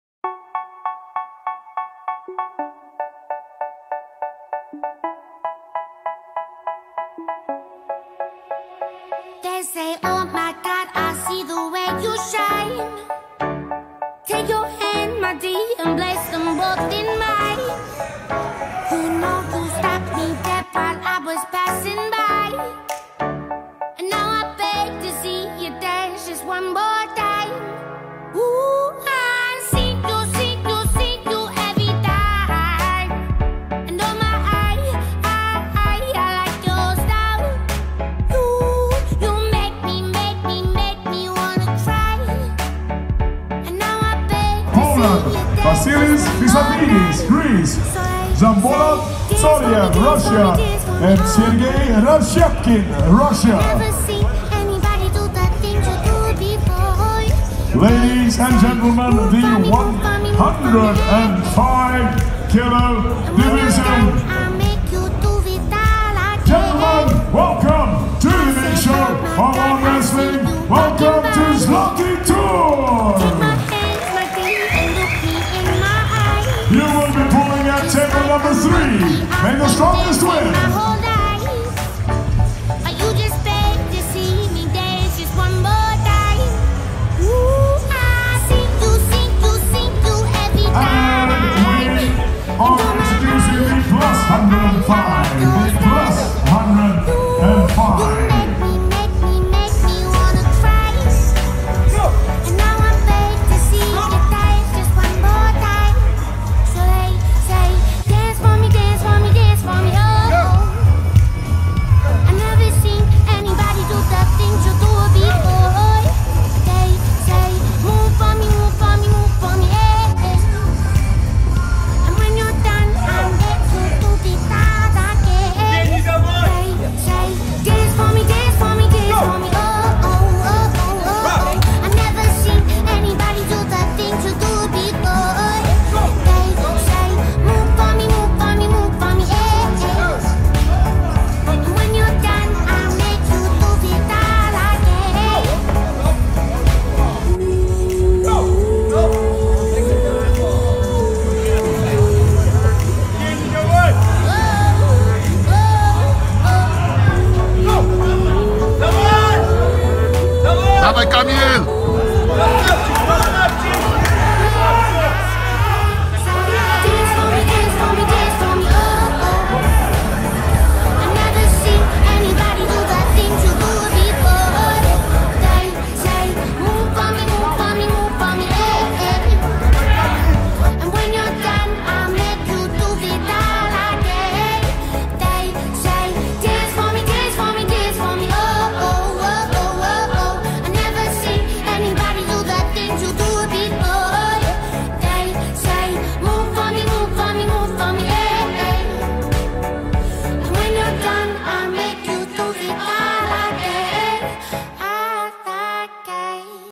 Zloty Tur 2019 one of the biggest armwrestling event - Kamil Jablonski Golden Dragon
my beginning in top competitions